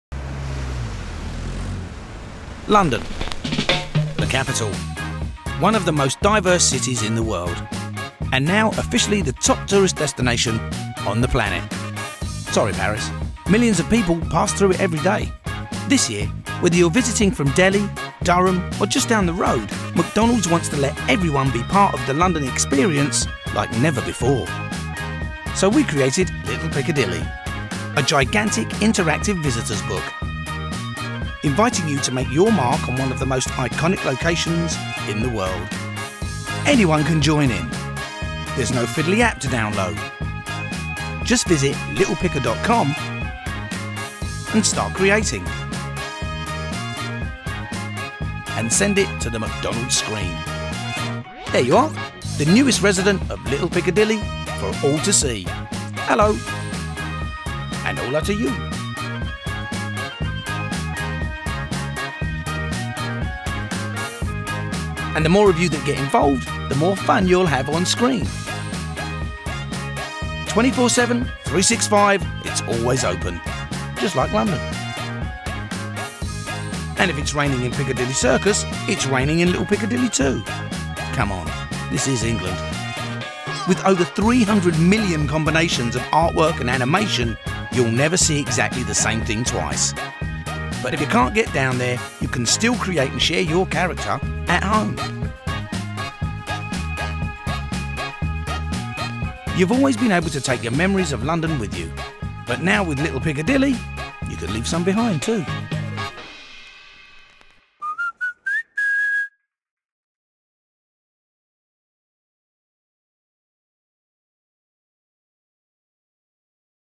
McDonald's Little Piccadilly
McDONALD’S SUPPORTS LITTLE PICCADILLY INITIATIVE IN AND AROUND PICCADILLY CIRCUS
Leo Burnett amplifies ‘Little Piccadilly’ around Piccadilly Circus to support new interactive experience for McDonald’s iconic Piccadilly sign